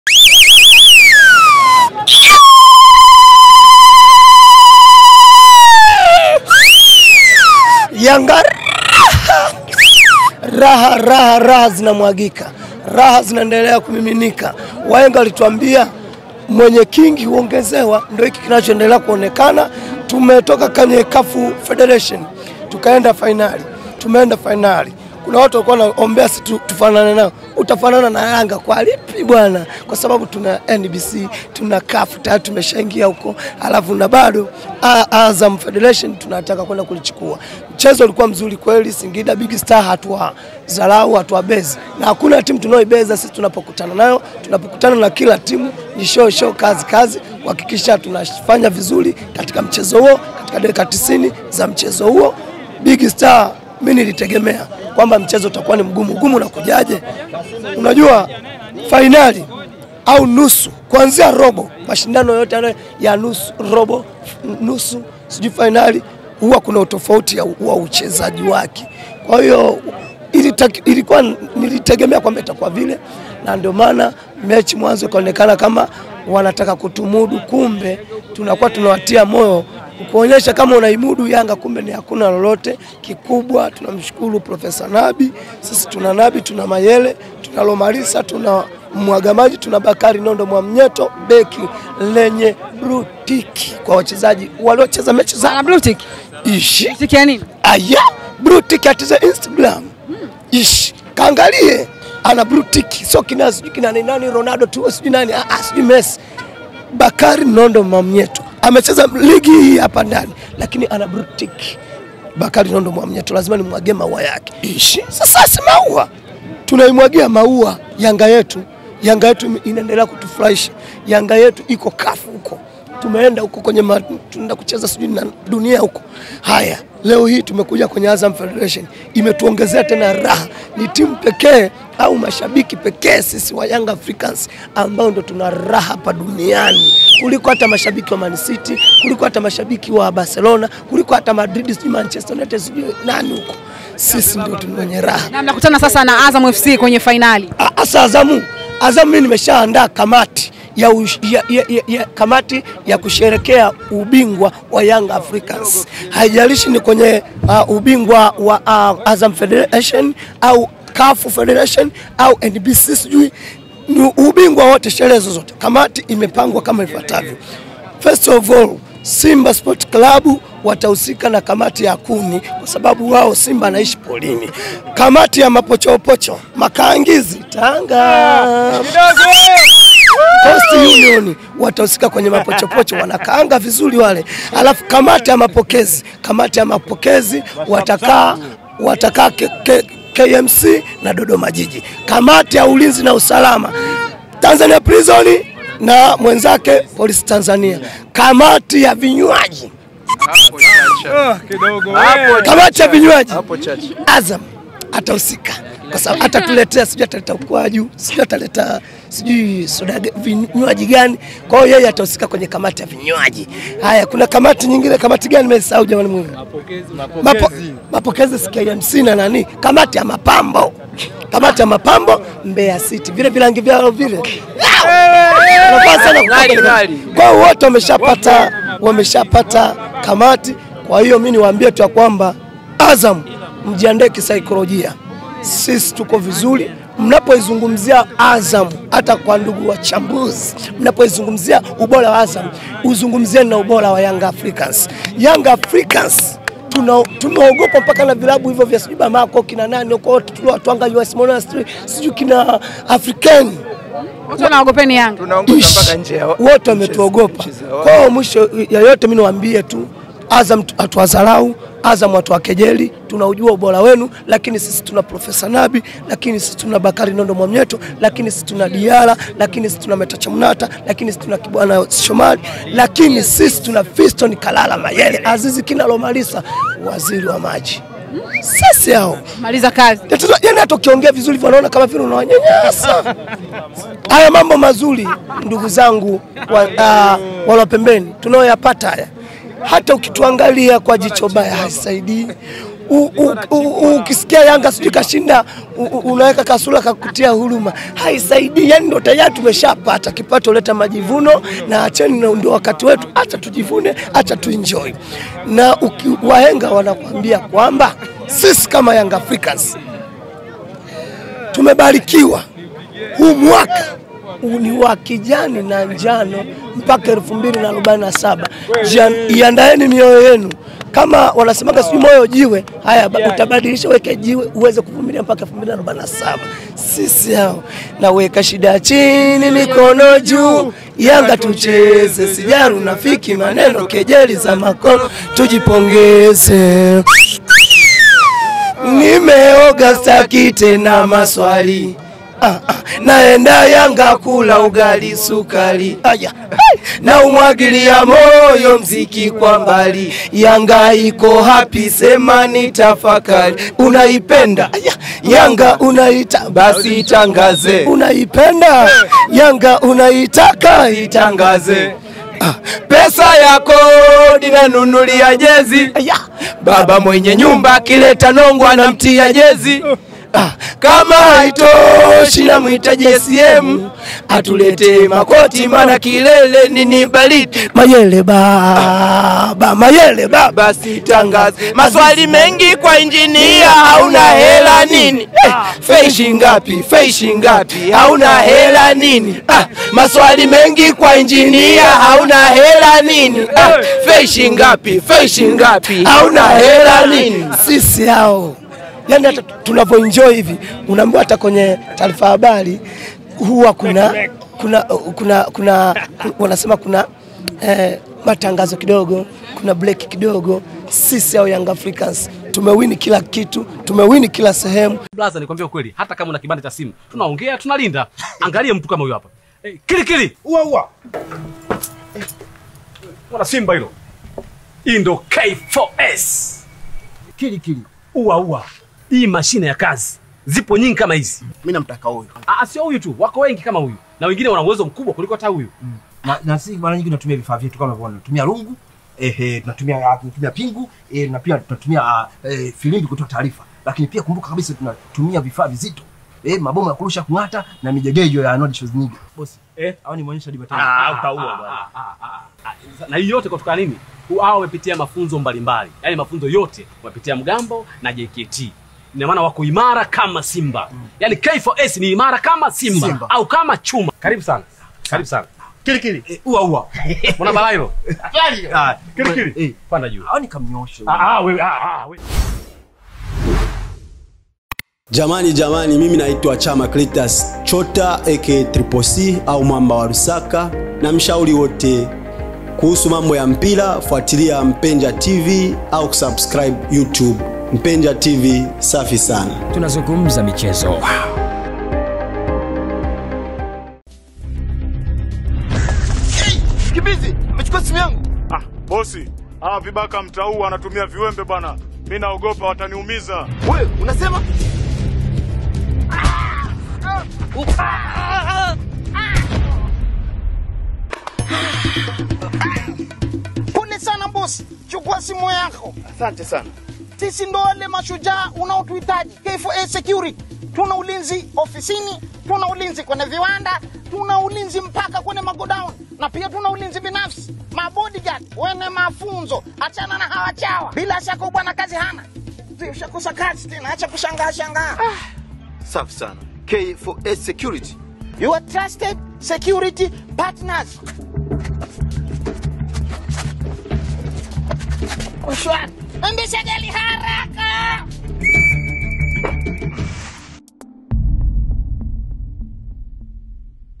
KIGELEGELE" SISI TUNA NABI NA MAYELE, HATUWEZI KUFANANA/ RAHA ZINAONGEZEKA/MWENYE KINGI HUONGEZEWA
Shabiki wa Yanga SC,Kigelegele ashusha tambo baada ya Yanga SC kutinga Fainali ya Kombe la Shirikisho la Azam sports .